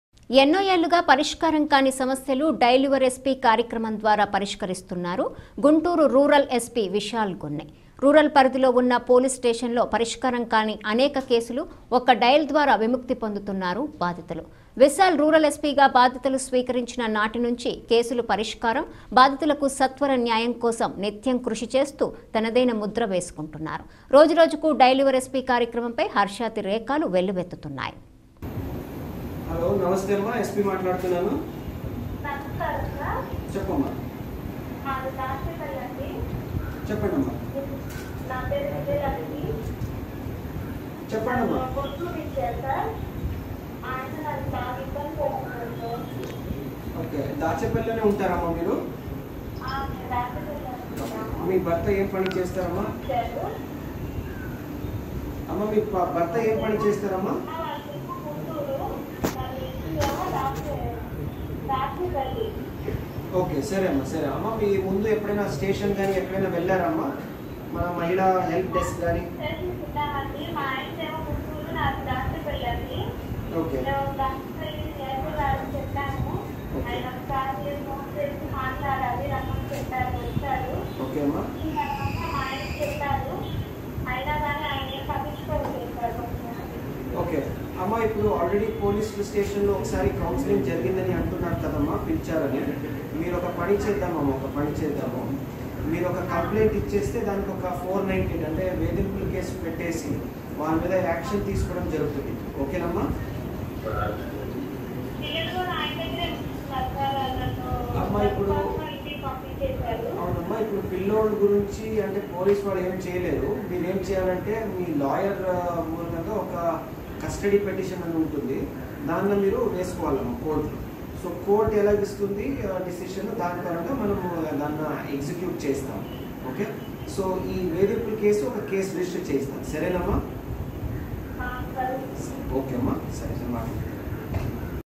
గుంటూరు రూరల్ జిల్లా ఎస్పీ విశాల్ గున్ని డయల్ యువర్ ఎస్పీ కార్యక్రమం...
ఎన్నోఏళ్లుగా  పరిష్కారం కాని సమస్యలు డయల్ యువర్ ఎస్పీ కార్యక్రమం ద్వారా పరిష్కరిస్తున్నారు గుంటూరు రూరల్ ఎస్పీ విశాల్ గున్నీ. రూరల్ పరిధిలో ఉన్న పోలీసు స్టేషన్ లో పరిష్కారం కాని అనేక కేసులు ఒక్క డయల్ ద్వారా విముక్తి పొందుతున్నారు బాధితులు. విశాల్ రూరల్ ఎస్పీగా బాధ్యతలు స్వీకరించిన నాటినుంచి కేసులు పరిష్కారం, బాధితులకు సత్వర న్యాయం కోసం నిత్యం కృషిచేస్తూ తనదైన ముద్ర వేసుకుంటున్నారు. రోజురోజుకు డయల్ యువర్ ఎస్పీ కార్యక్రమంపై  హర్షాతిరేకాలు వెల్లువెత్తుతున్నాయి.